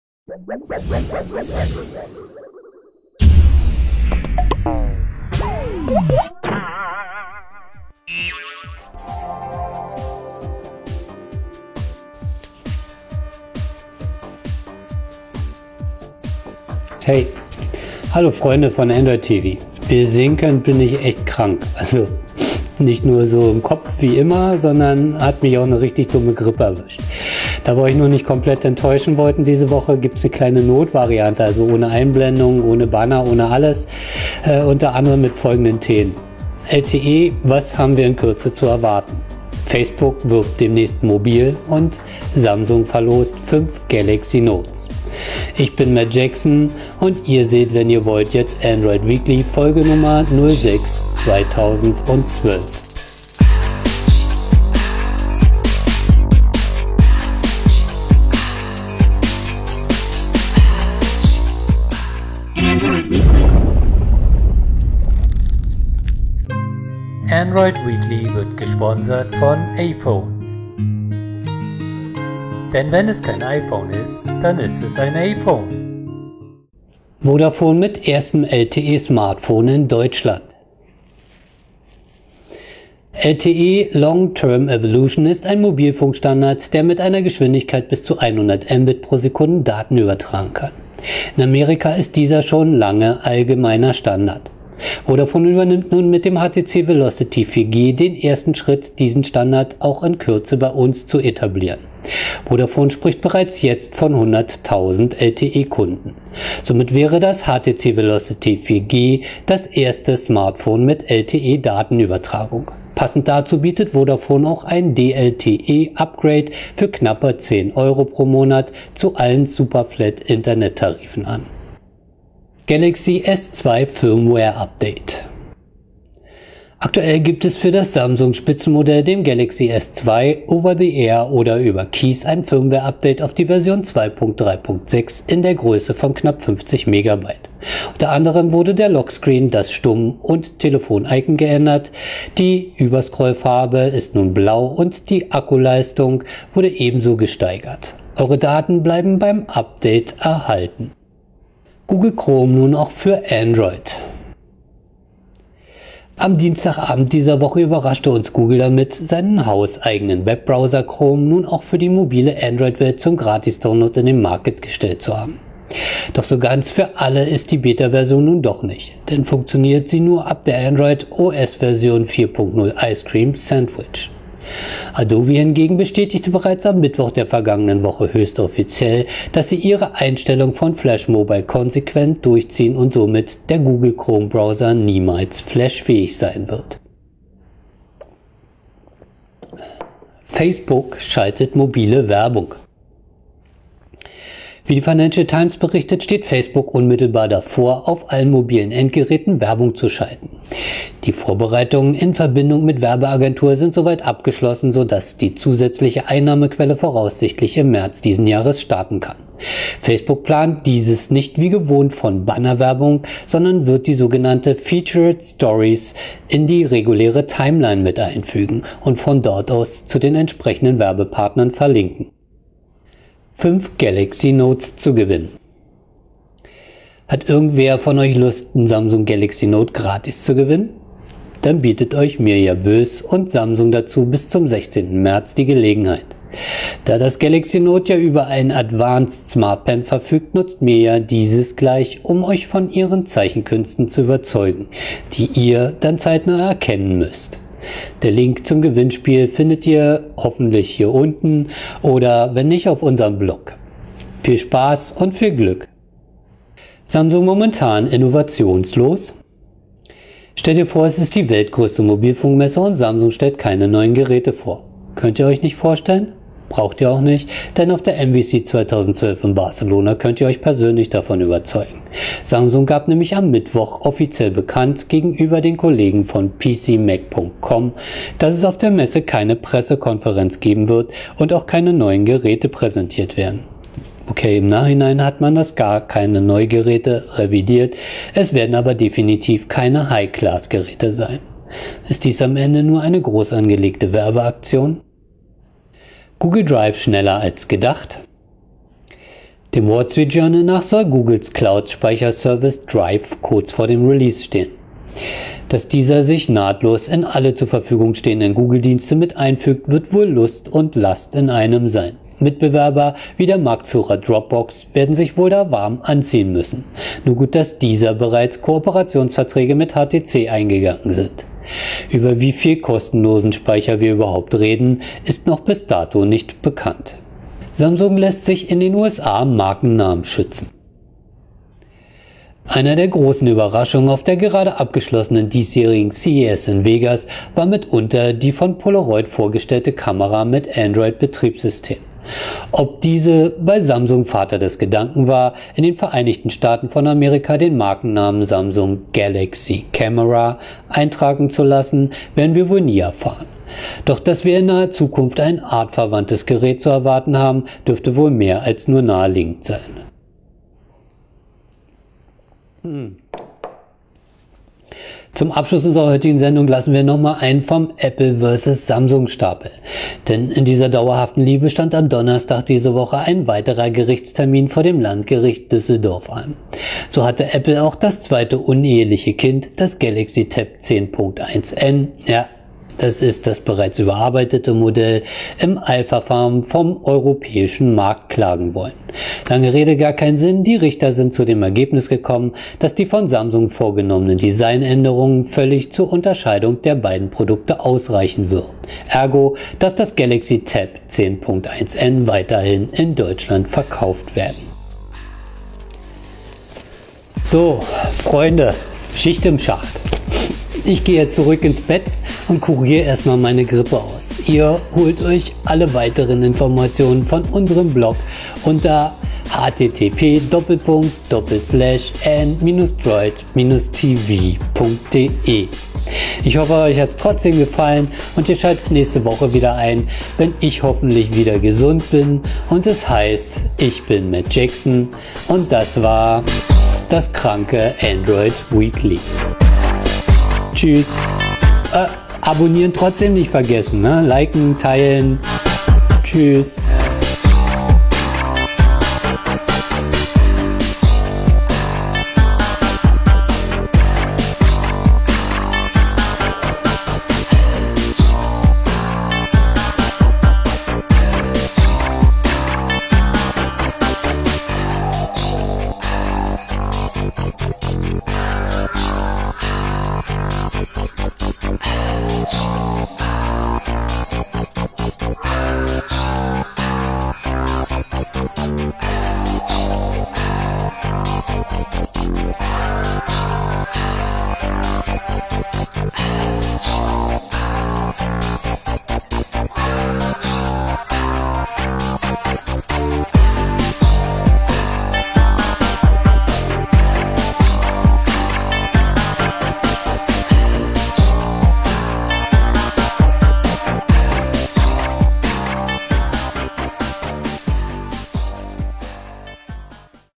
anDROID weekly - Folge Nr.06-2012
anDROID weekly ist die Erste Produktion von anDROID TV.
Die Sendung stellt euch die Highlights der vergangenen Woche aus der Welt des anDROID Smartphone und Tablet vor.

Unter anderem mit diesen Themen:

- was haben wir demnächst von LTE zu erwarten?

- Facebook demnächst mit Werbung auf mobilen Endgeräten

und

- 5 Samsung Galaxy Note zu gewinnen!

im Auftrag von: anDROID TV
Moderator: MaTT Jackson